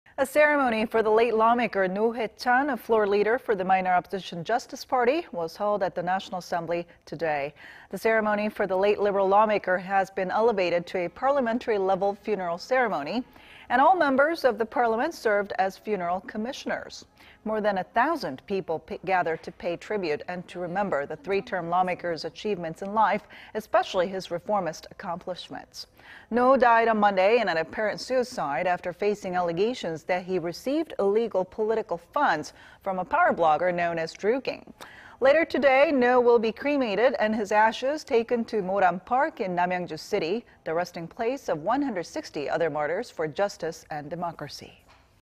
Funeral ceremony for former floor leader of Justice Party held on Friday
국회, 오늘 故 노회찬 의원 영결식 거행
  A ceremony for the late lawmaker Roh Hoe-chan, a floor leader for the minor opposition Justice Party, was held at the National Assembly on Friday.
  The ceremony for the late liberal lawmaker has been elevated to a parliamentary-level funeral ceremony,... and all members of the parliament served as funeral commissioners.
More than a thousand people gathered to pay tribute and to remember the three-term lawmaker's achievements in life, especially his reformist accomplishments.
Roh died on Monday, in an apparent suicide after facing allegations that he received illegal political funds from a power blogger known as "Druking".
 Later today,... Roh will be cremated and his ashes taken to Moran Park in Namyangju City, the resting place of 160 other martyrs for justice and democracy.